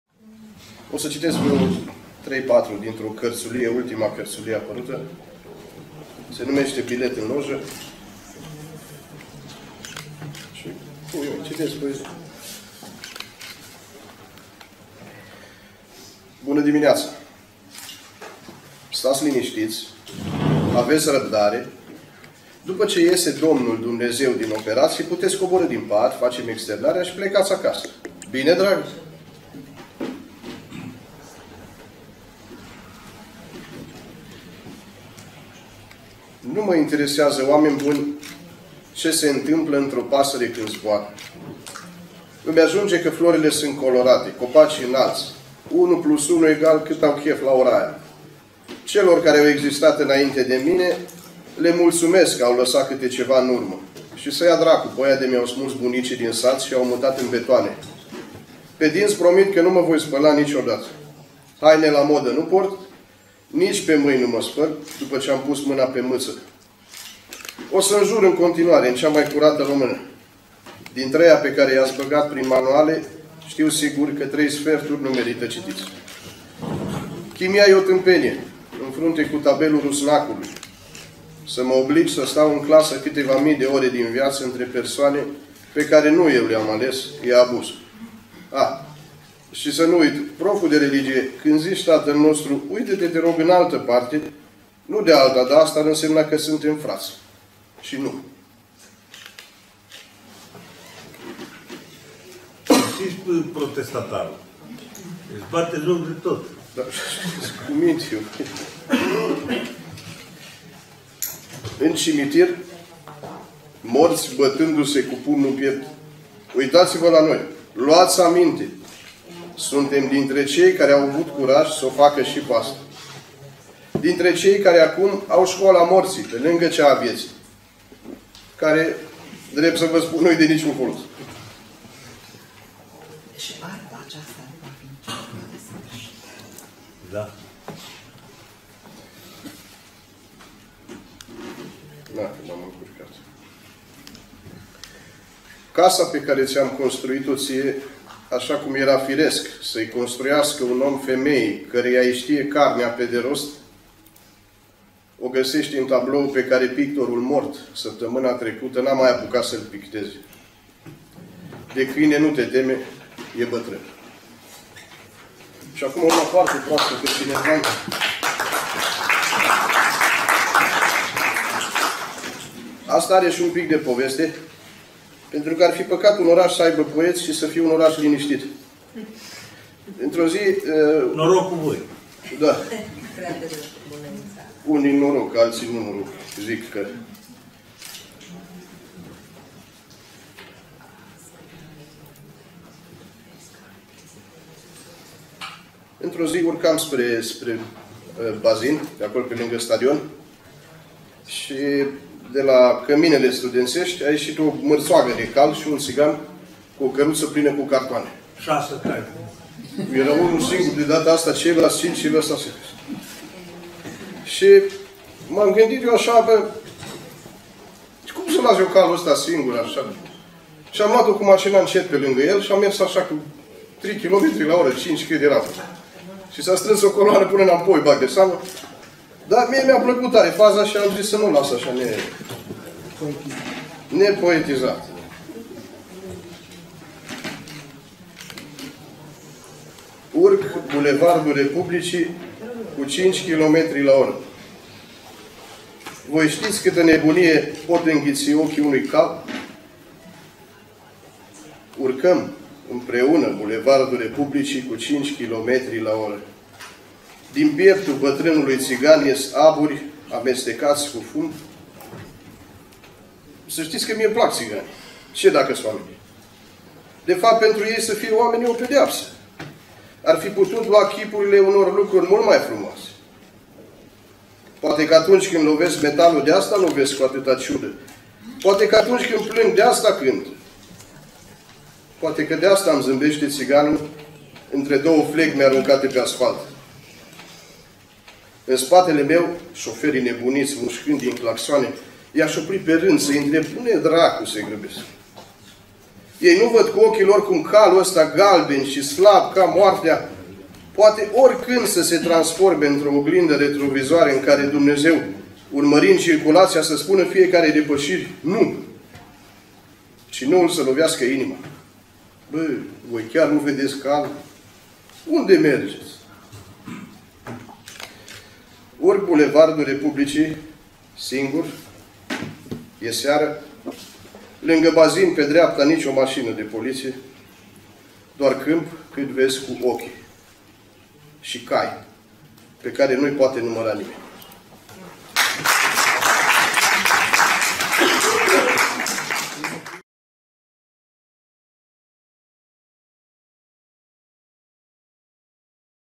SILVIU DACHIN - COLOCVIILE TINERILOR SCRIITORI CONFERINTELE DE POEZIE DE LA ALBA IULIA - 2016
Consiliul Judetean Alba

Uniunea Scriitorilor di Romania Filiala Alba-Hunedoara

Biblioteca Judeteana "Lucian Blaga" Alba

COLOCVIILE TINERILOR SCRIITORI
CONFIRENTELE DE POEZIE DE LA ALBA IULIA

Partecipa tinerii:

RALUCA LUCIA CIMPEAN

alaturi de invitati speciali din Italia:

RAFFAELE D'ORAZI
FABRIZIO CARRIERI
RICCARDO SPINELLA

Program:
17 octobrie 2016
- ora 10,00 Aula Mica a Uneversitatii "1 Decembre 1918" di Alba Iulia

Partner:
Universitatea "1 Decembrie 1918" din Alba Iulia

IAMU S.A. Blaj